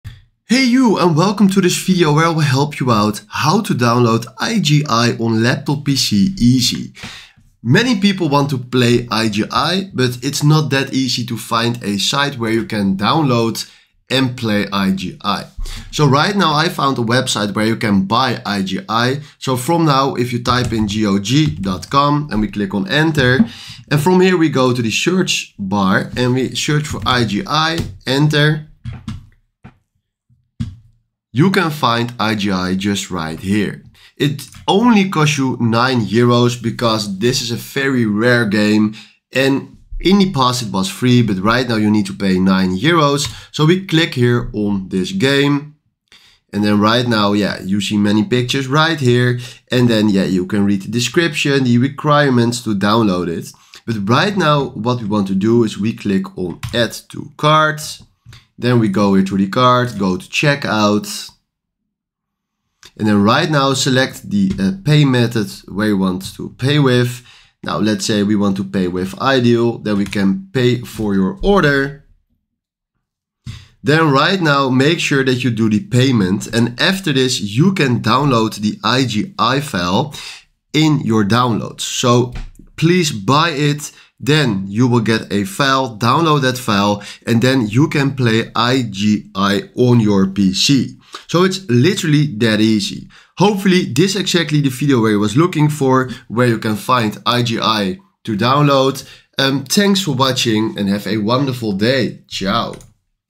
How to Download IGI on Laptop/PC (Easy)
Learn how to download and install Project I.G.I on your laptop or PC easily. This step-by-step 2025 guide walks you through the full setup process so you can enjoy this classic tactical shooter without errors or complicated settings. Works on Windows 10/11.